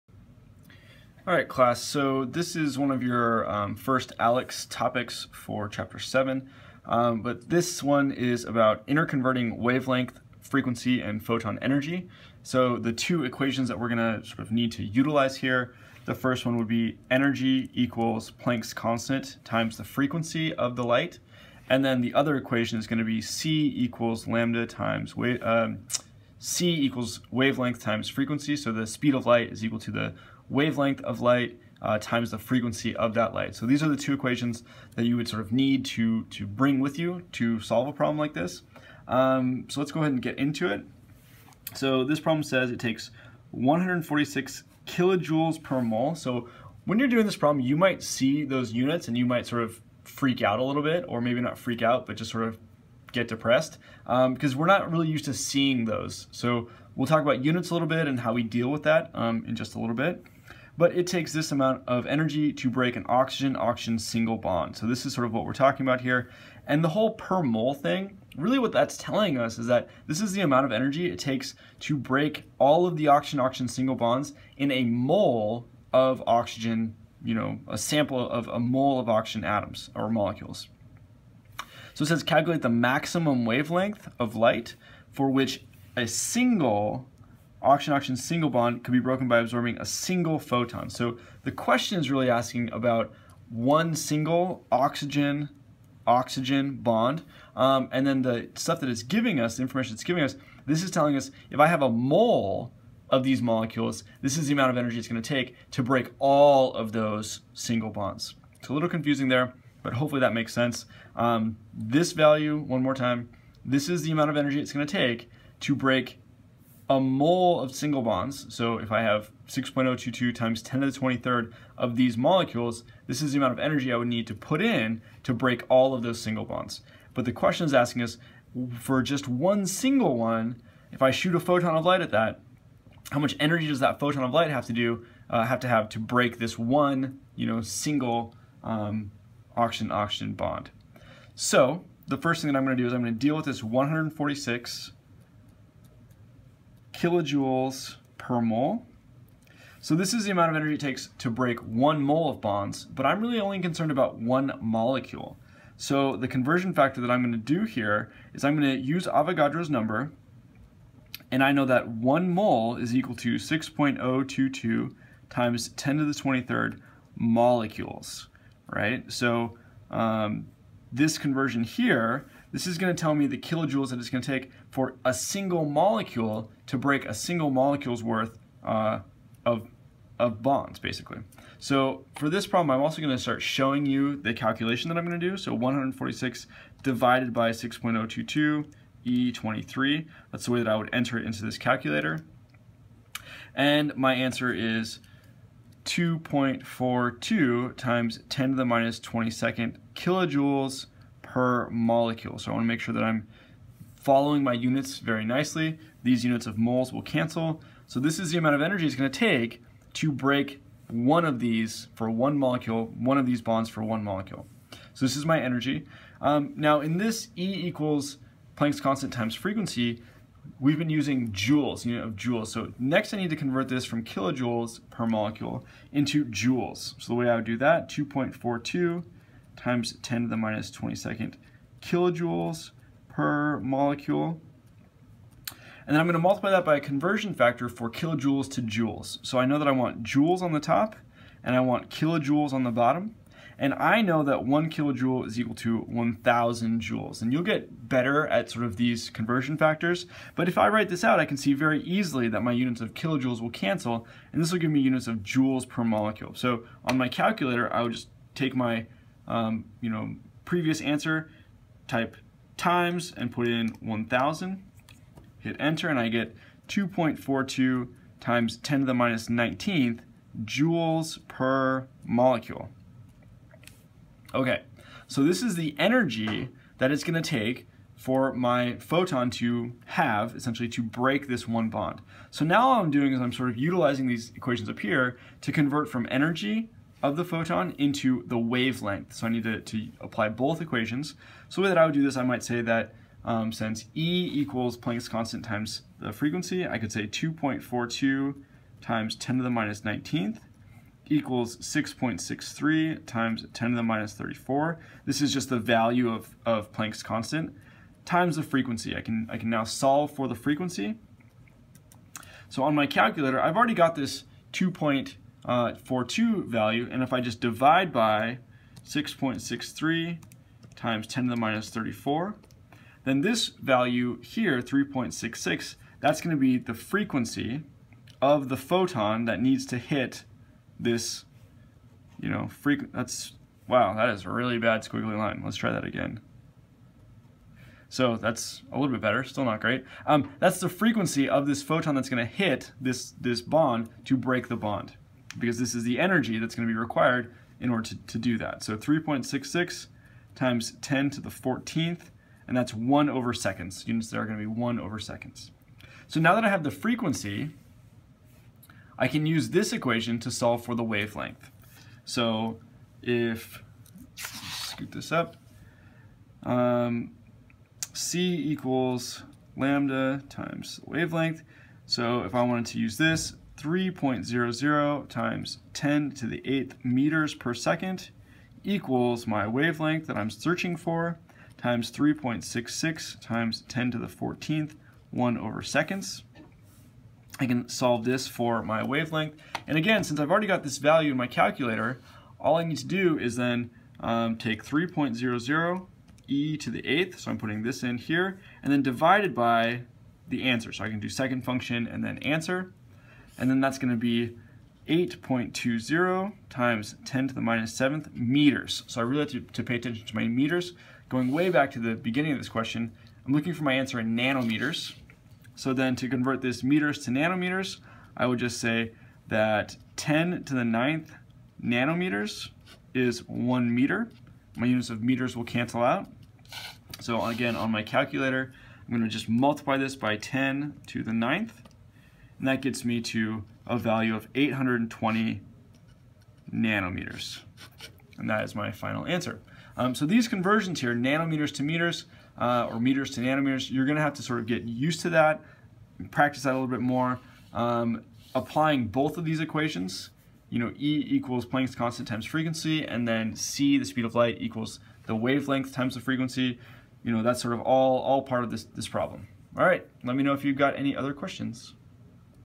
ALEKS - Interconverting Wavelength, Frequency, and Photon Energy
This video shows how to calculate the wavelength of light that would be required to break an oxygen-oxygen bond.